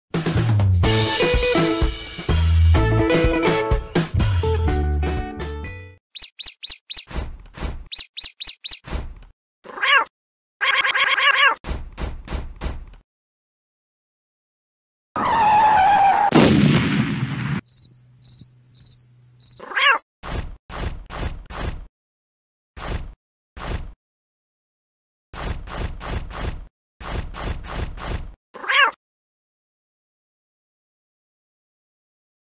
The Blue-Cat & The Pink-Mouse: "Sign Language" (Ep. #28)
¡hi viewers! welcome to this new episode of The Blue-Cat & The Pink-Mouse titled "Sign Language", in this episode, Blue-Cat learns sign language (ASL).